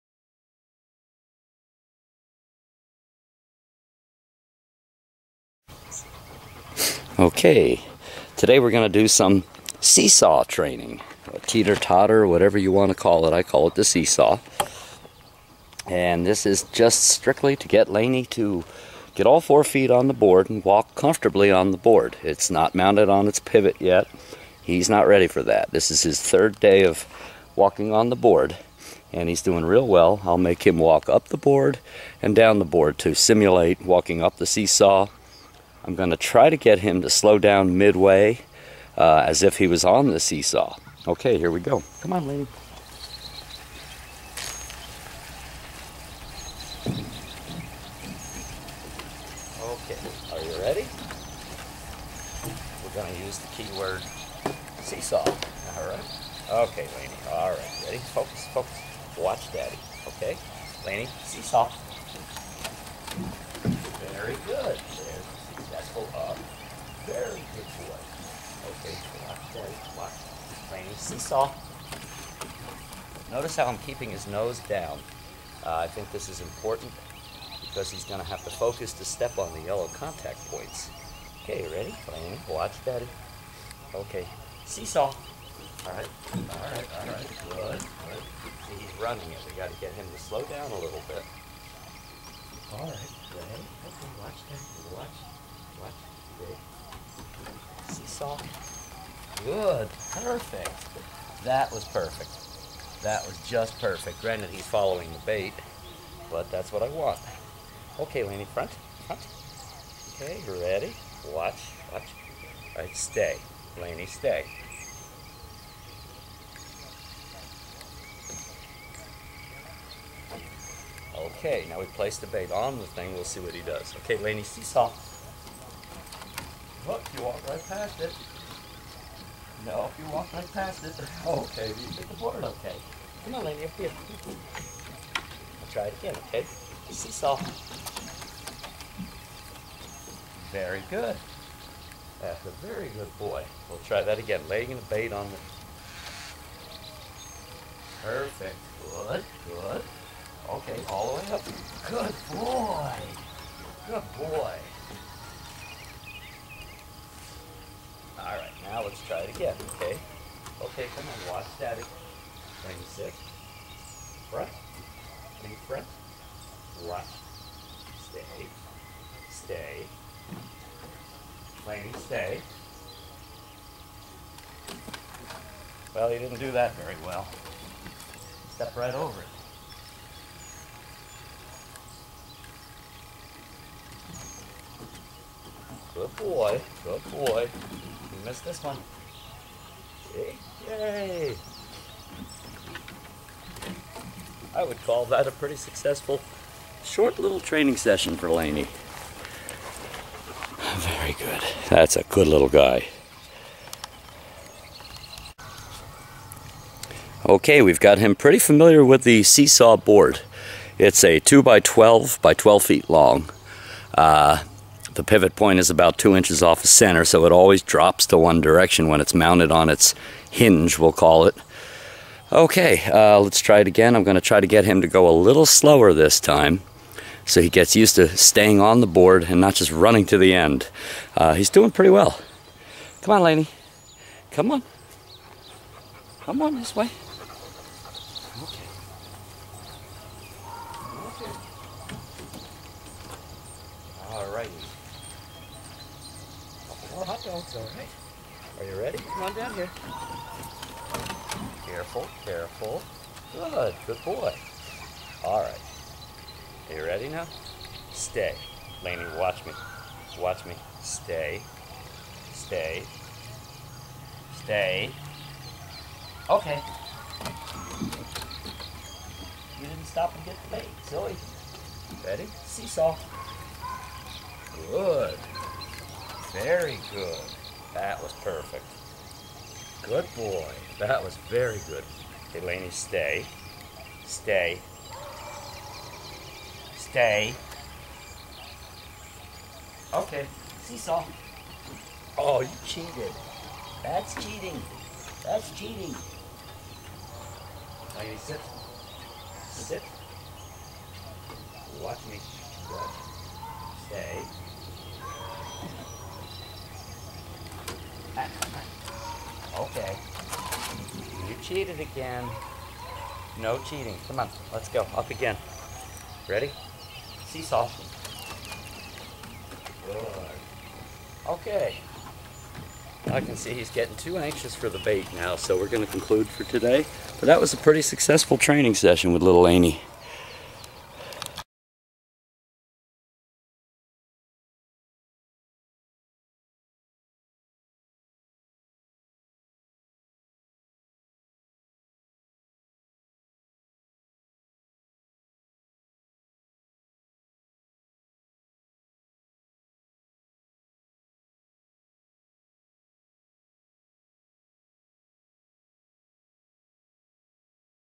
Canine Agility intro. to SeeSaw
"Laine" the almost 11 month old Tri-color Rough Collie being introduced to the seesaw. In this video Laine is learning to get comfortable walking on a 12" wide seesaw board while keeping Laine's safety as the main factor. (no seesaw base yet)